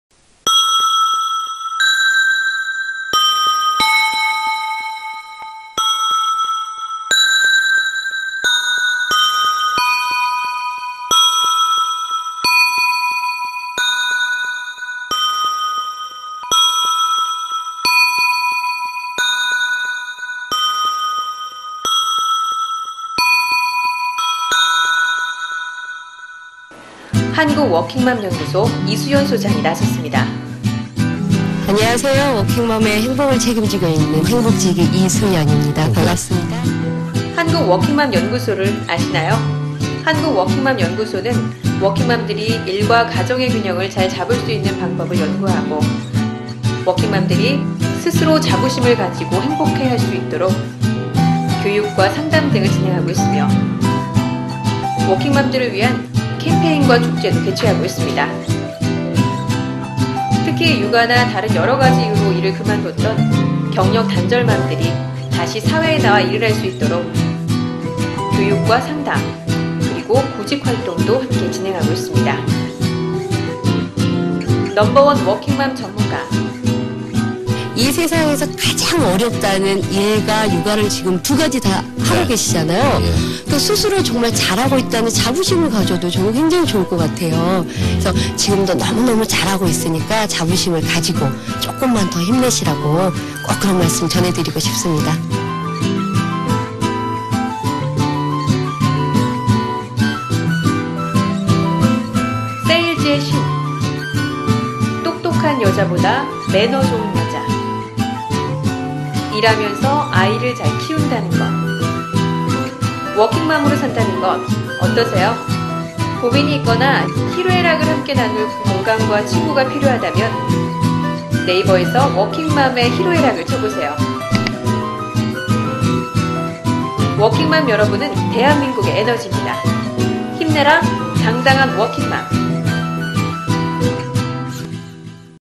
[장스프로덕션] 한국워킹맘연구소 이수연 소장 홍보영상 - PD 장형관
마음 속 감동의 영상제작소, [문화창작집단]장스프로덕션에서 한국워킹맘연구소 이수연 소장님 홍보영상(광고,CF)를 제작하였습니다. 내레이션은 '톡톡 감동산책'의 차원진 MC가 참여하였습니다. 대한민국 워킹맘들의 행복을 책임지는 이수연 소장님의 행보에 박수를 보내드립니다. 힘내라, 당당한 워킹맘!! ^^

- 장형관 PD -